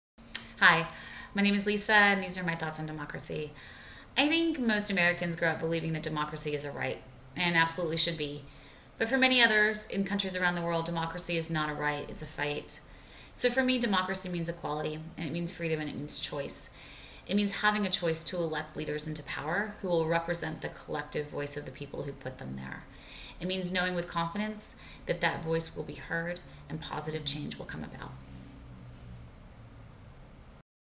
They Will be Heard_Lisa Dulyea
Los Angeles, CA - Lisa Dulyea shares her feelings on Democracy as part of They Will be Heard.